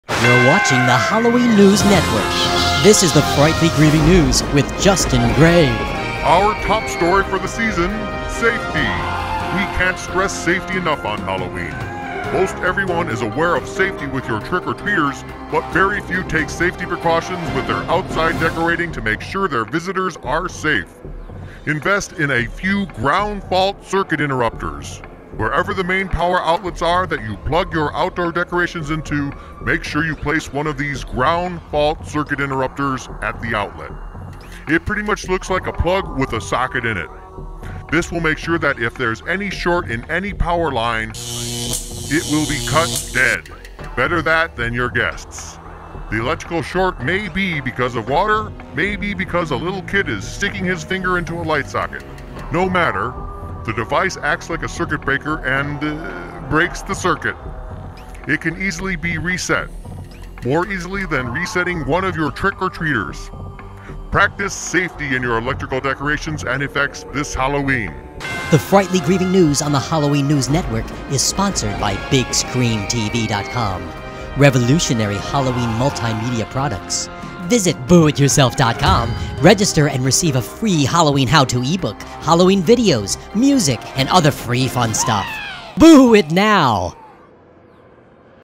Halloween Decoration Safety - Electrical Precautions
Many people to extra care and safety while trick-or-treating, but few consider the safety of their guest trick-or-treaters. Make sure your outdoor electrical decorations shut off immediately if shorted out.